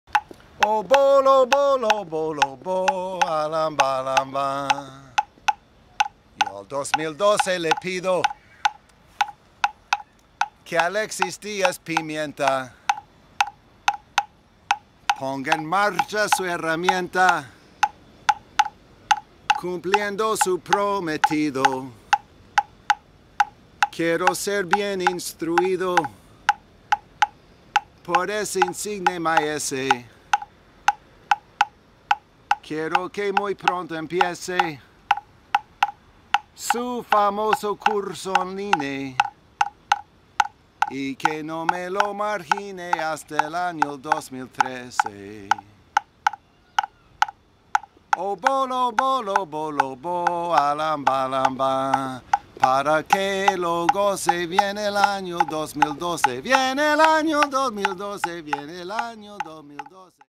Año Nuevo, Verso Nuevo
Para Casa América y mi amigo Alexis

Al 2012 le pido
que Alexis Díaz Pimienta
ponga en marcha su herramienta
cumpliendo su prometido.
Quiero ser bien instruído
por este insigne maese
quiero que muy pronto empiece
el famoso curso on-line
y que no me lo margine
hasta el año 2013.